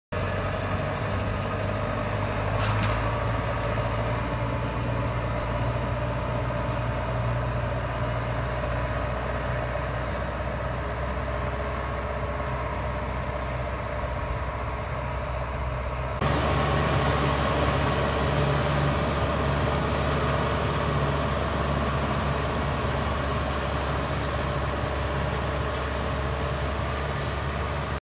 트랙터로 밭갈이 하기
연일 계속 내리는 비로 인하여 밭을 만들수가 없다
해서 궁여지책으로 물빠짐을 위해 밭을 갈아본다
풀뿌리농장 석성수